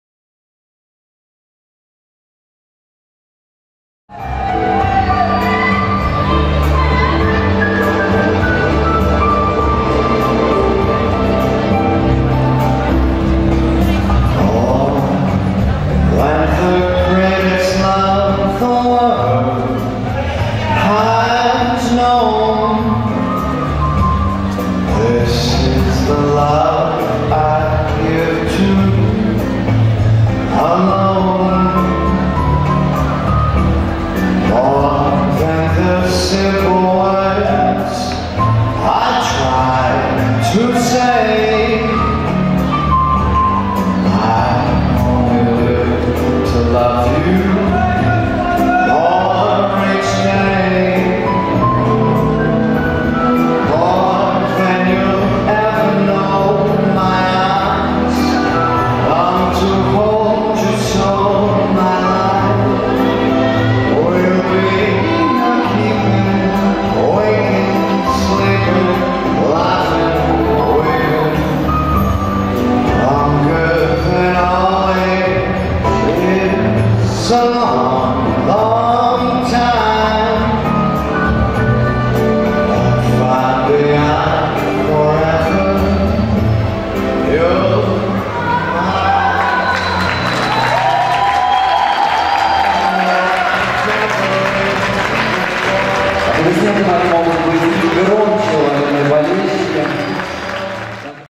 Родыгин Александр - Чиненкова Александра, Финал, Румба
Чемпионат Приволжского федерального округа
Нижний Новгород, 26-27 января